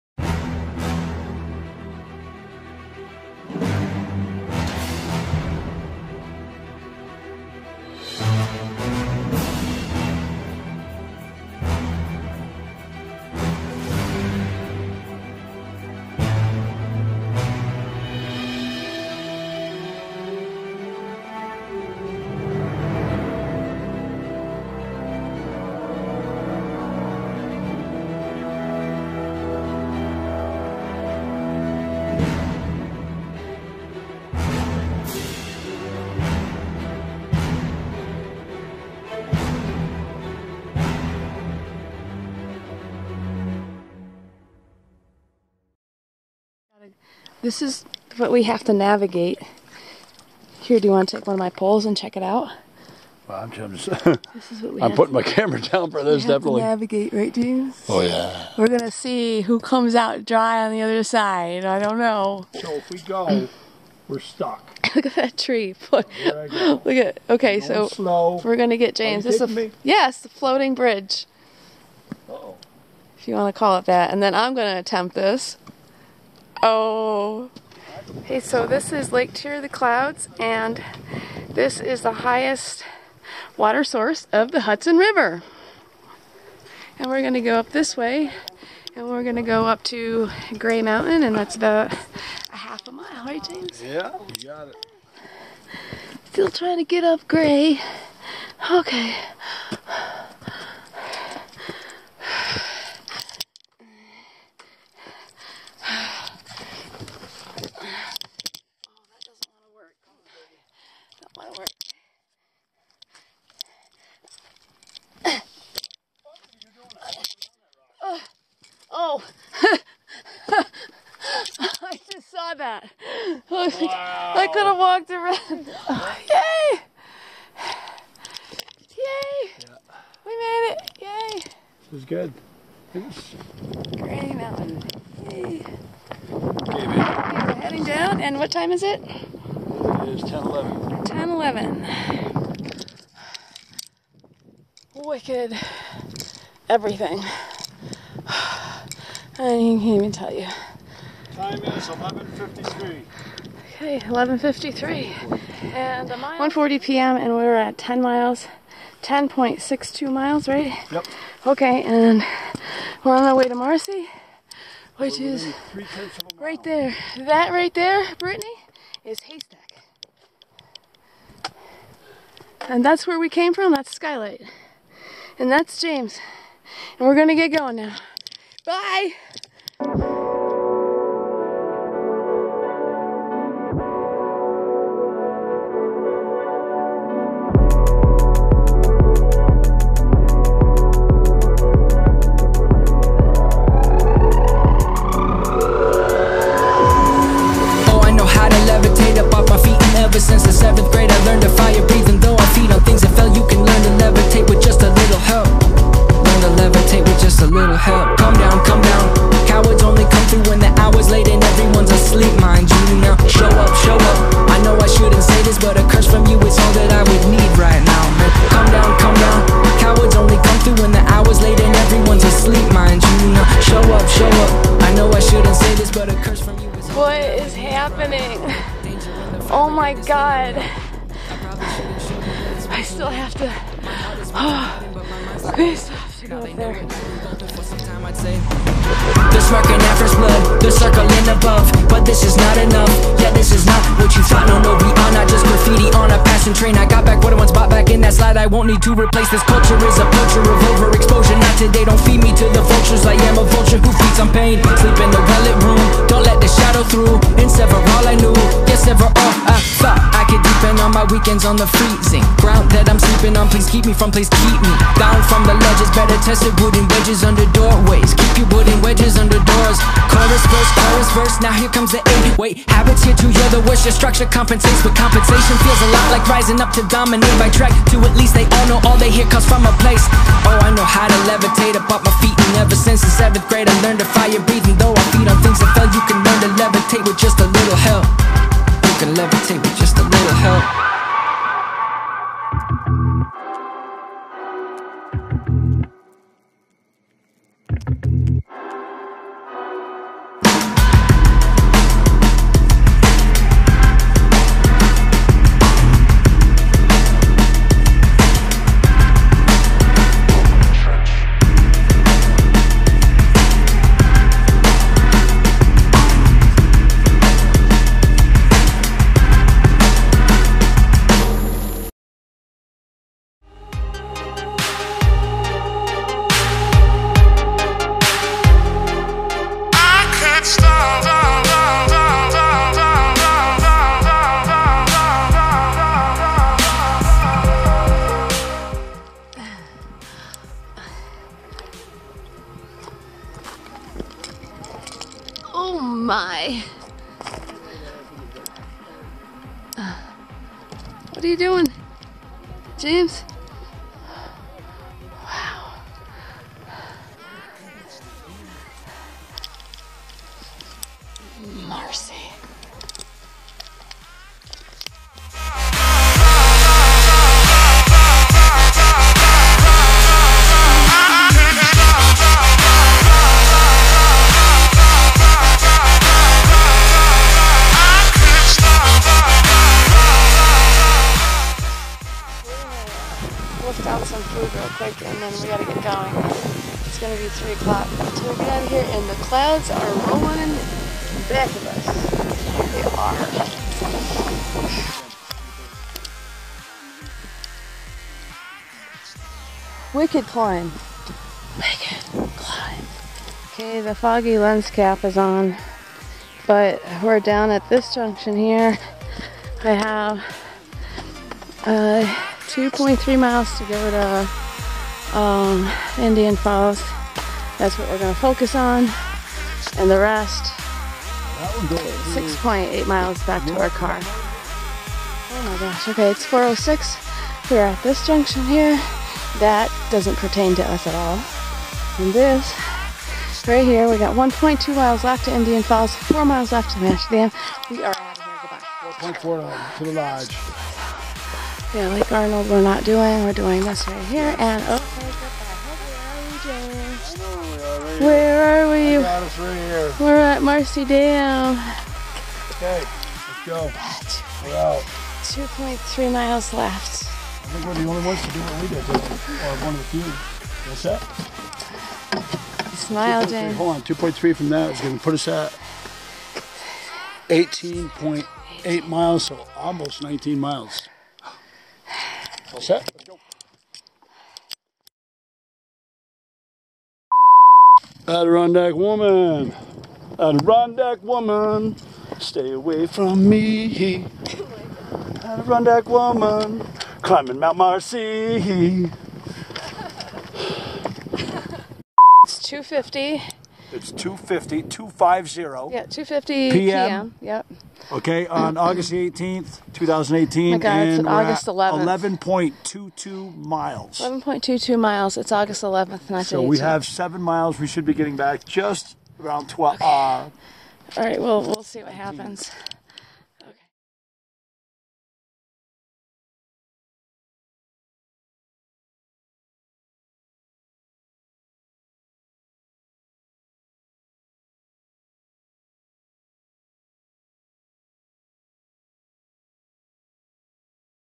Mt. Marcy, Mt. Skylight, and Gray Peak - ADK Vlog #14
I'm so glad to finally have this vlog up for everyone to see. These 3 mountains were no simple feat but something to be entirely proud of. With beautiful views, interesting obstacles, and tackling the Marcy monster, the footage was a lot of fun to edit and we hope you enjoy the highlights. - Britt